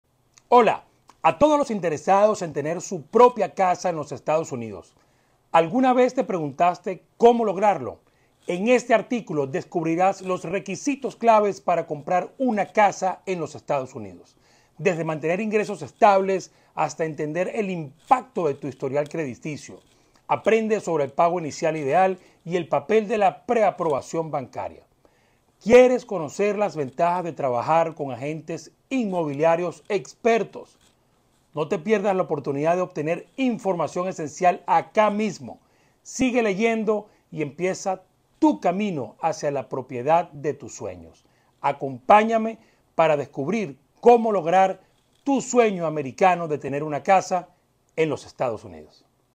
Como comprar casa en Estados Unidos. Preguntas Frecuentes.
Por: Rafael Zuzolo

Hola a todos los interesados en tener su propia casa en Estados Unidos

¿Alguna vez te preguntaste cómo lograrlo? En este artículo, descubrirás los "Requisitos Clave para Comprar una Casa en EE.UU."

Desde mantener ingresos estables, hasta entender el impacto de tu historial crediticio. Aprende sobre el pago inicial ideal y el papel de la pre aprobación hipotecaria.

𝙍𝙖𝙛𝙖𝙚𝙡 𝙕𝙪𝙯𝙤𝙡𝙤
𝙏𝙪 𝙍𝙚𝙖𝙡 𝙀𝙨𝙩𝙖𝙩𝙚 Broker 𝙚𝙣 𝙈𝙞𝙖𝙢𝙞
𝙋𝙧𝙤𝙪𝙙 𝙢𝙚𝙢𝙗𝙚𝙧 𝙤𝙛 𝙍𝙚𝙖𝙡𝙩𝙮 𝙊𝙉𝙀 𝙂𝙧𝙤𝙪𝙥 𝙀𝙫𝙤𝙡𝙪𝙩𝙞𝙤𝙣